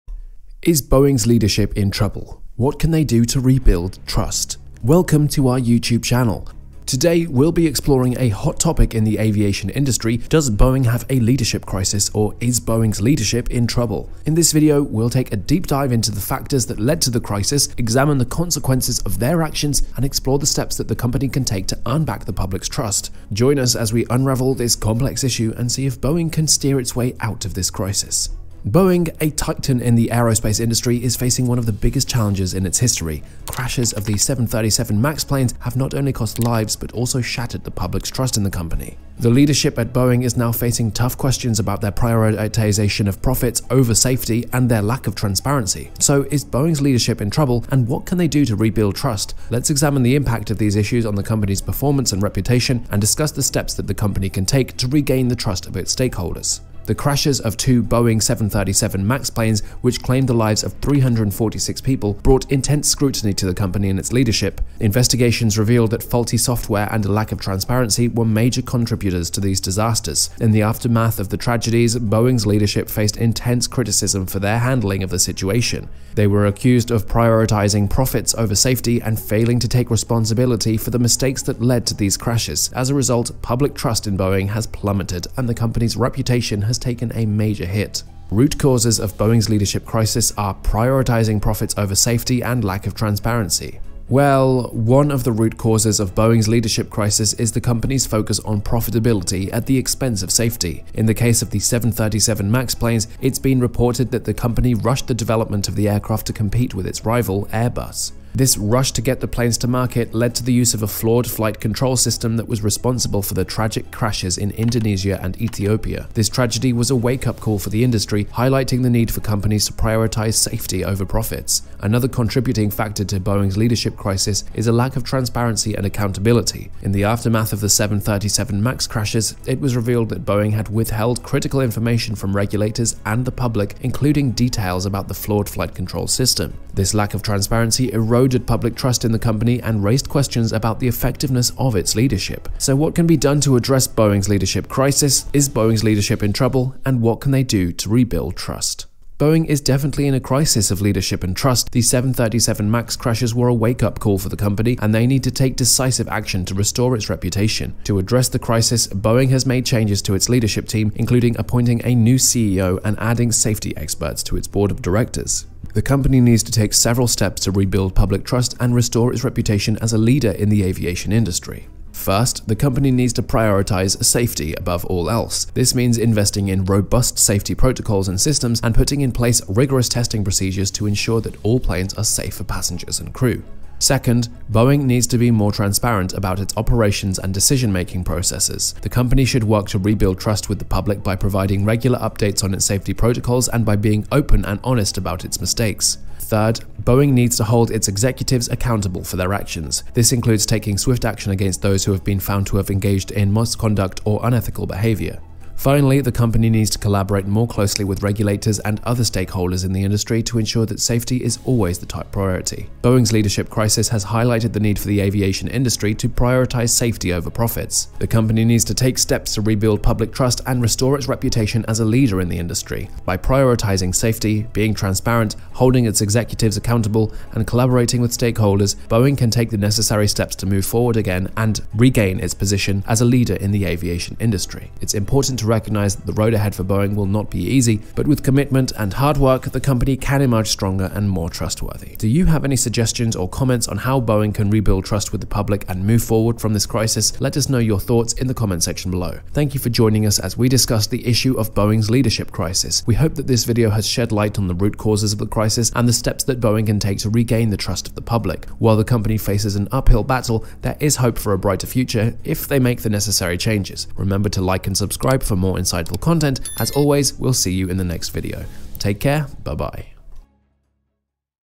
Is Boeing's Leadership In Trouble?
We'll be exploring a hot topic in the aviation industry Does Boeing have a leadership crisis or Is Boeing's leadership in trouble? In this video, we'll take a deep dive into the factors that led to the crisis, examine the consequences of their actions, and explore the company's steps to earn back the public's trust. Join us as we unravel this complex issue and see if Boeing can avoid this crisis.